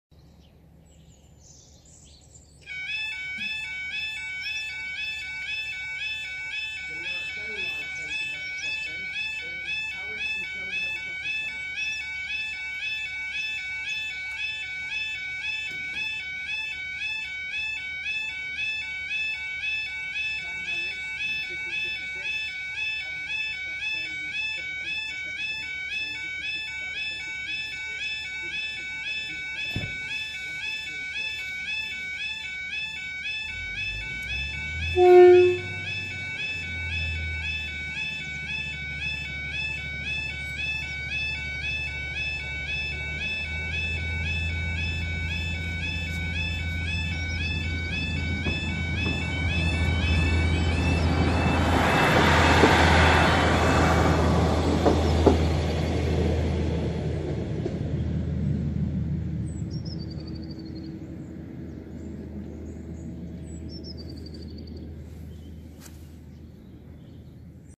Dolau Level Crossing (Powys) 17.02.2018
Crossing Type: AOCL+B.
Line: Swansea to Shrewsbury.
Trains Per Hour: 1.
Lights: 4 Unipart Rail LED Wigwags.
Alarms: 2 Clifford and Snell Yodel Alarms.
Number of Tracks: 1.
Postcode: LD1 6UP.
Train Operating Company: Arriva Trains Wales.